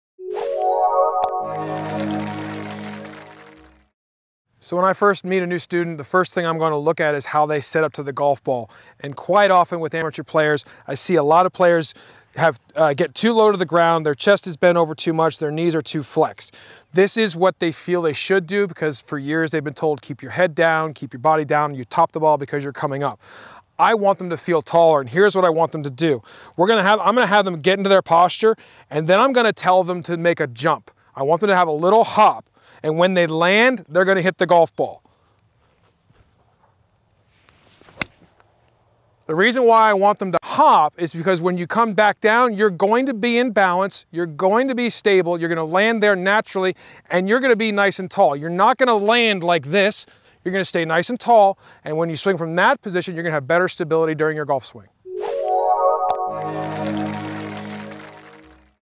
How To Feel Tall at Address
Keeping your chest up and staying tall in your posture is necessary for you to turn efficiently. This drill while looking odd will help you feel the height needed in your set up.